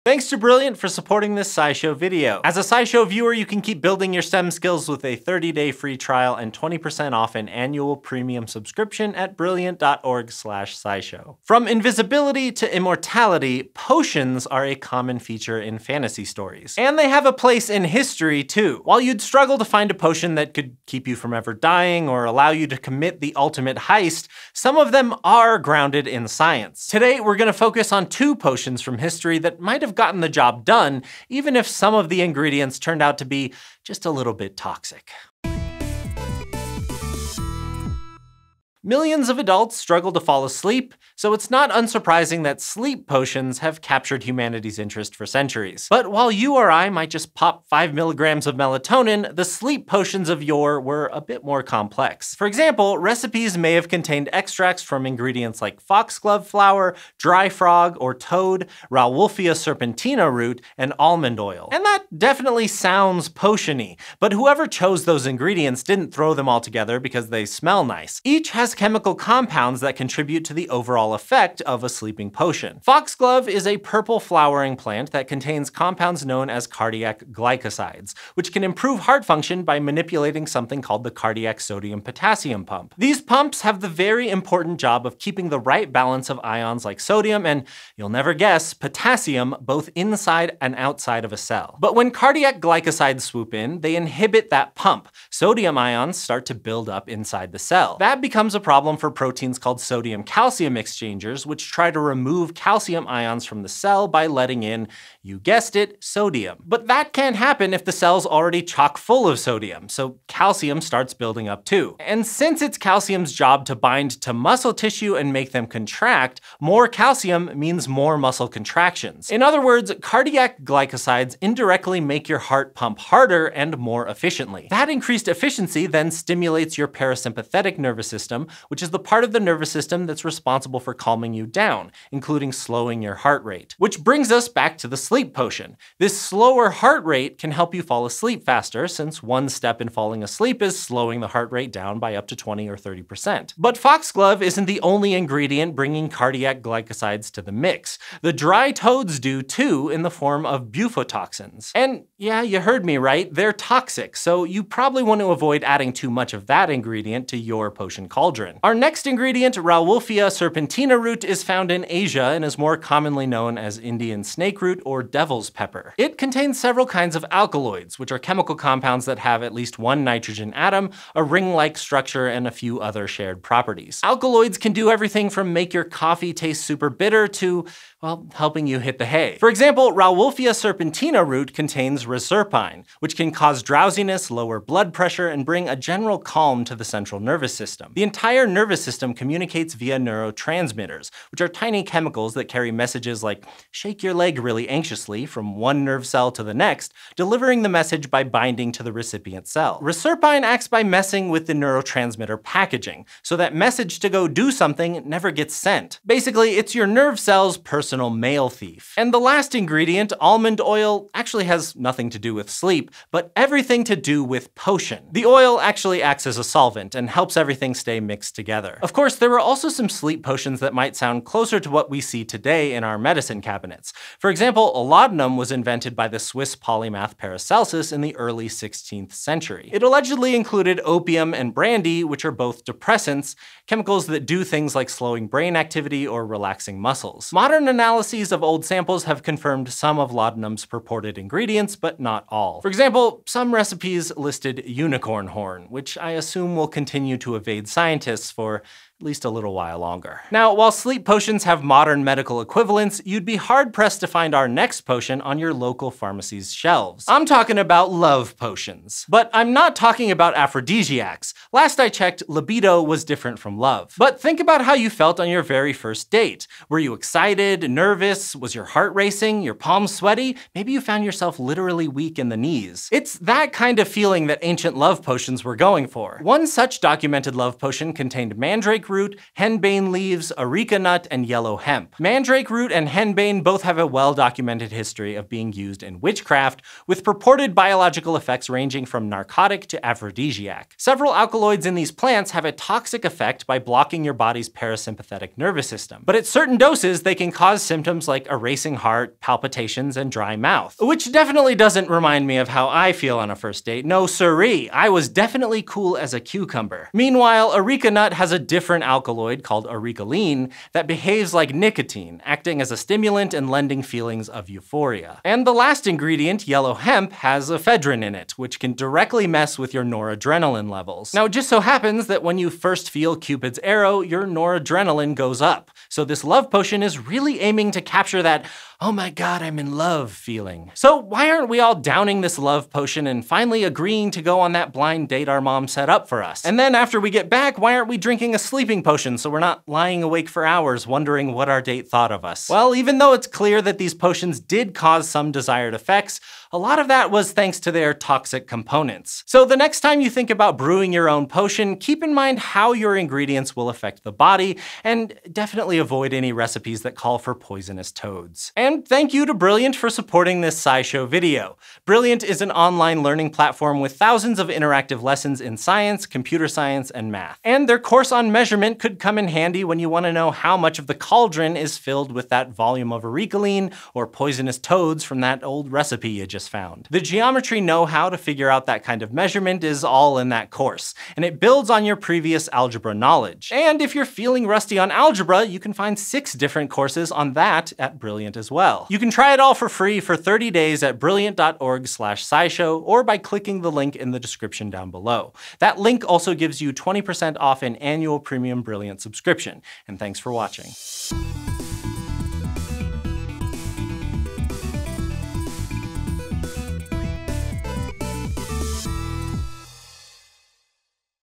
The Science Behind Sleep & Love Potions
Sure, potions of invisibility and immortality may be a little hard to come by in the real world, but there's some legit science behind less fantastic ones. Historical sleep and love potions are grounded in science, even if some of the ingredients are a bit toxic.

Images